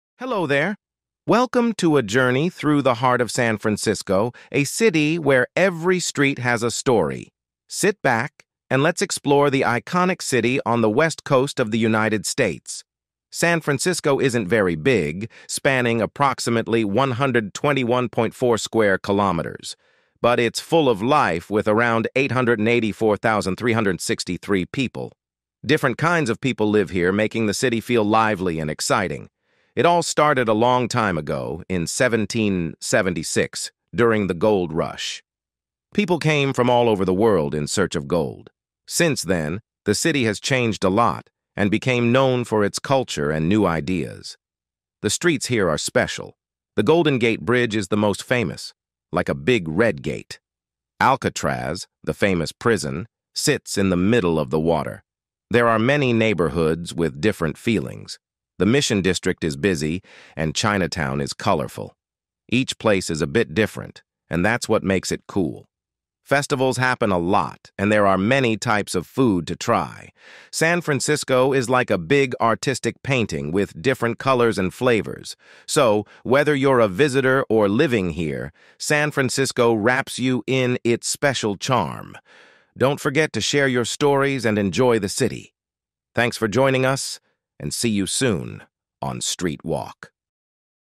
San Francisco: Explore the History and Culture of an Iconic City 🌉
Immerse yourself in the essence of San Francisco with this immersive video that delves into its fascinating history and vibrant culture. Discover well-kept secrets, from picturesque alleyways to iconic neighborhoods. Join us for a captivating journey through time and space in this unique metropolis. 🏙️🔍